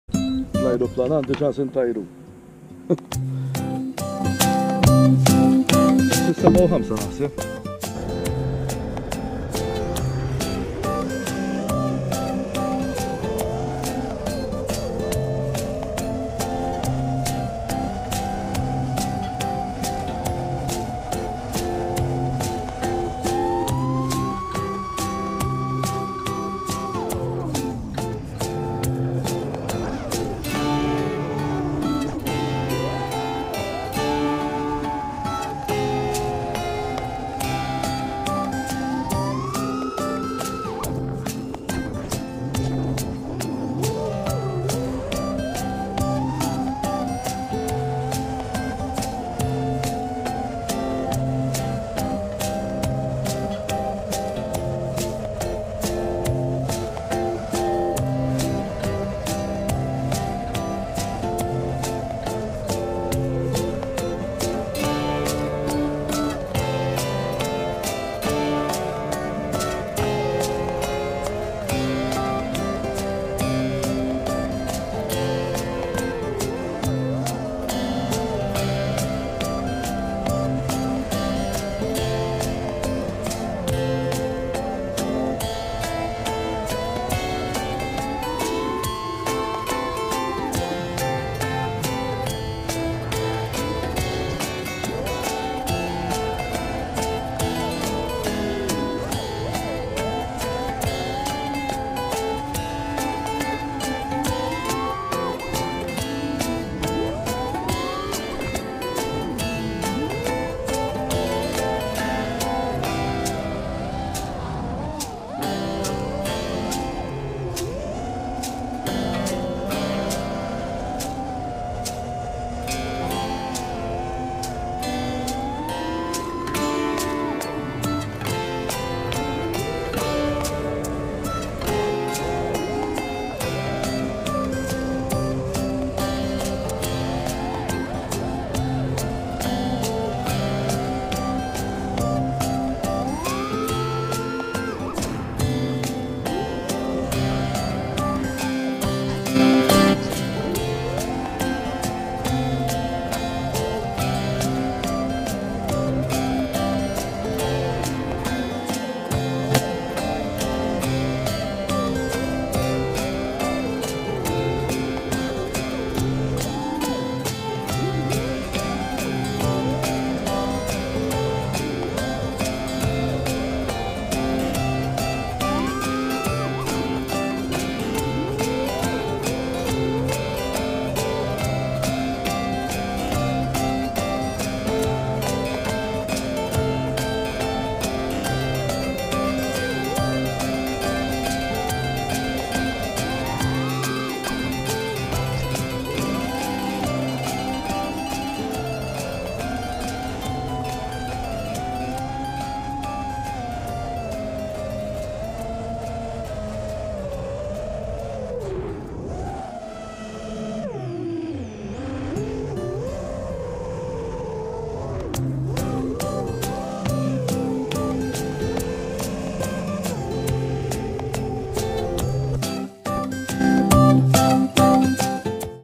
Early morning session
Just enjoying the smooth flight of my Armattan chameleon ti before doing some LOS flying with my Dynam Waco 🙏🙏🛫✈️